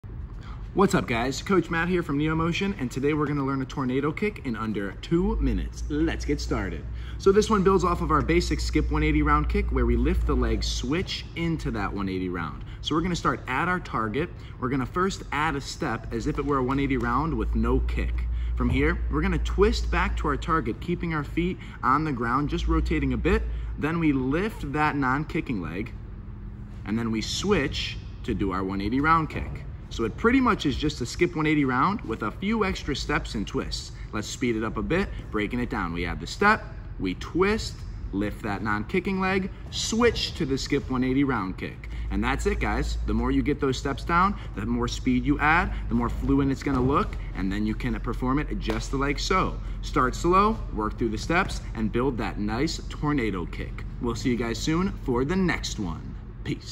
How to Do a Tornado Kick - Learn Tricking
Here we demonstrate the tornado kick, another Taekwondo style kick, which builds on the 180 round kick and skip 180 round kick from previous videos. For a challenge, make a combo with tornado kick, 180 hook kick, cartwheel, and dive roll!

Home Tumbling Mats
Kicking Targets

Always make sure you are properly warmed up and have adequate space for your training. Be smart and know your limitations.